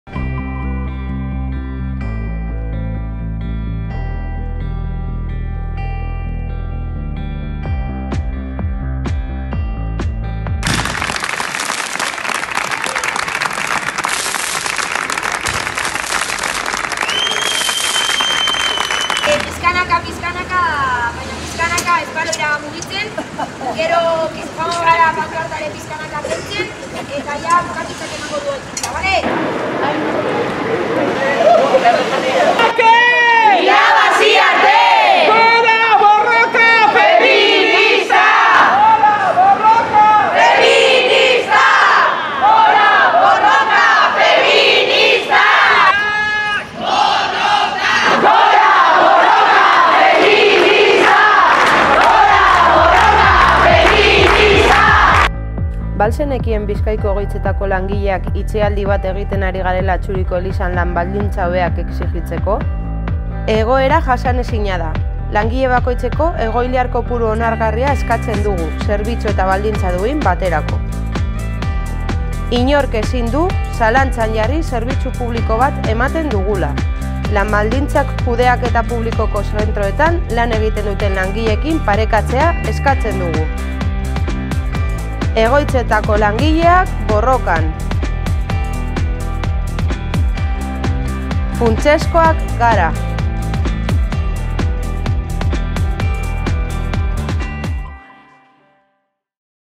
Itxialdia amaituta, borrokan aurrera
3.greba egunarekin amaitu da Bizkaiko erresidentzietako LABeko emakume grebalarien itxialdia. Txalo artean hartu dituzte irteeran. Ekimena amaitu arren borrokan jarraitzen dute.